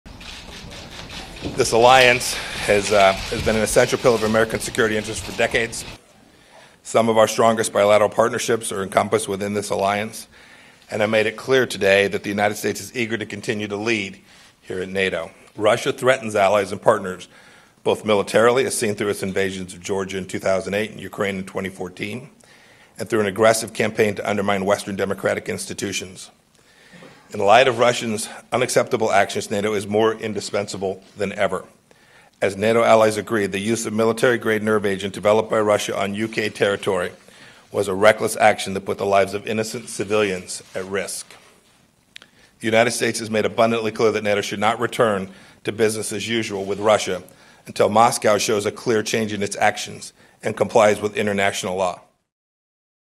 Pompeo: NATO Shouldn't Cooperate With Russia
Newly inducted United States Secretary of State Mike Pompeo had a clear message for NATO members regarding Russia. He said "NATO should not return to business as usual with Russia until Moscow shows a clear change in its actions." (April 27)